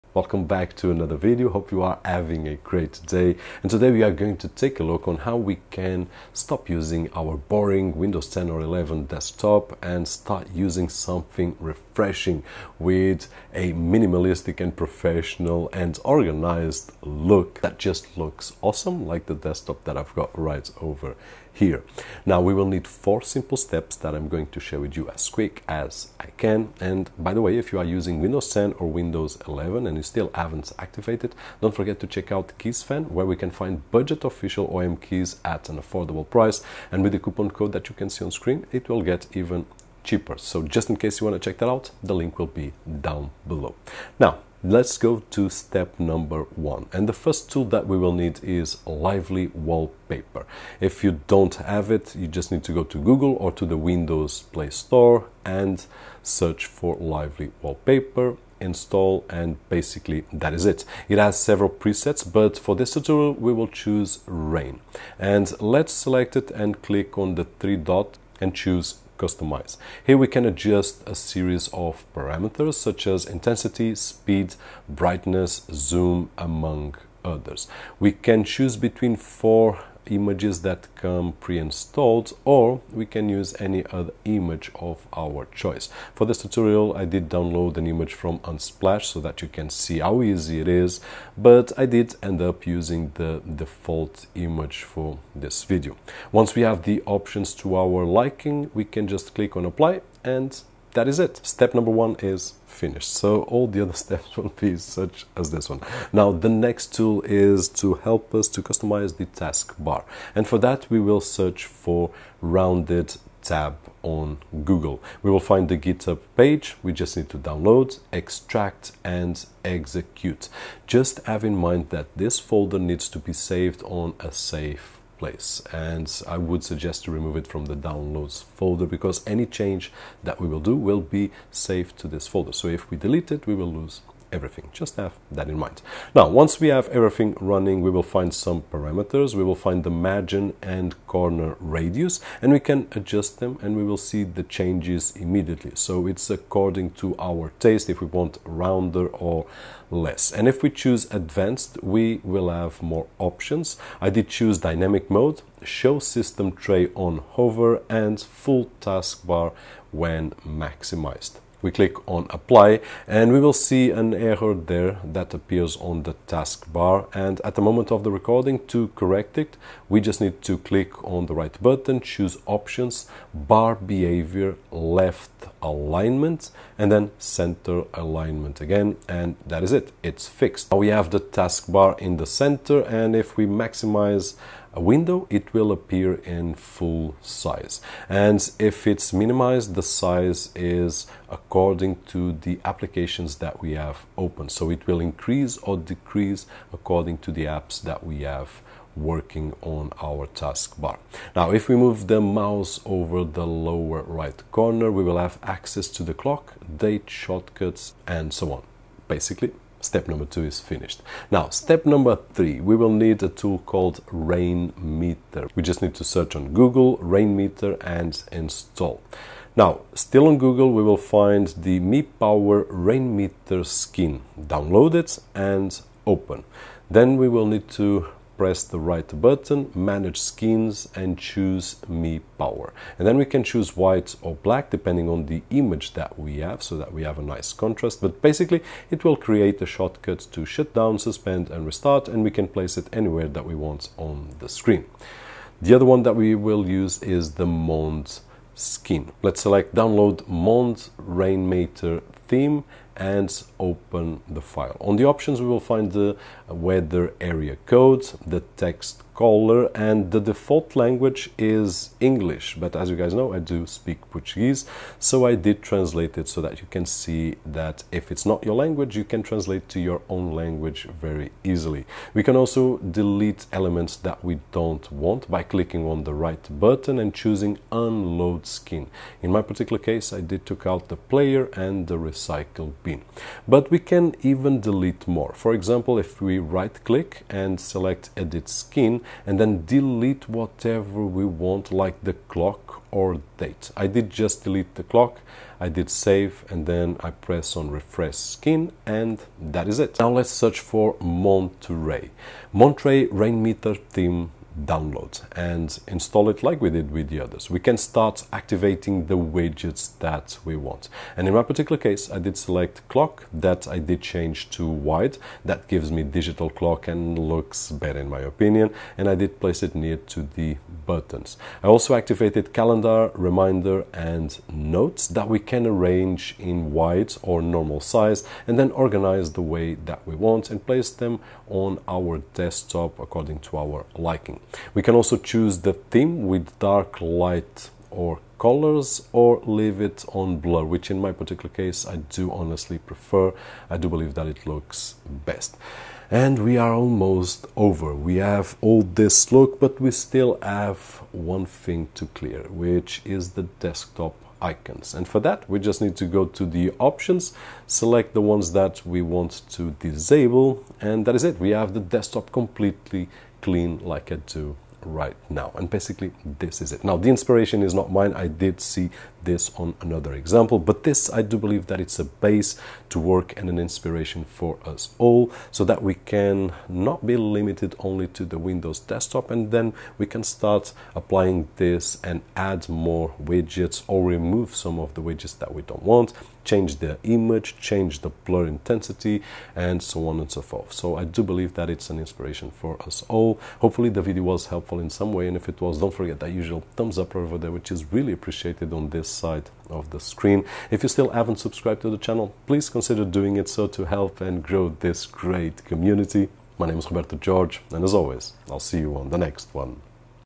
Make Your DESKTOP Minimalistic, Professional & Clean UNDER 7 Minutes !
Today we are going to turn our Windows 11 desktop into a minimalist, clean and attractive desktop with a professional and organized look in just 7 minutes
Windows 10 Pro at $7 (50%: RJV50)

✅ RECORDING GEAR: ✅

00:00:00 Intro
00:00:49 1st Lively Wallpaper
00:01:50 2nd RoundedTB
00:03:32 3rd Rainmeter
00:05:52 4th Removing Icons
00:06:05 Conclusion